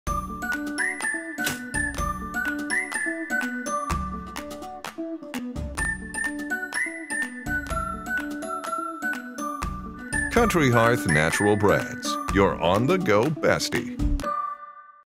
Peanut Butter and Jelly Time
An American classic; peanut butter and jelly on Kid's Choice White Bread.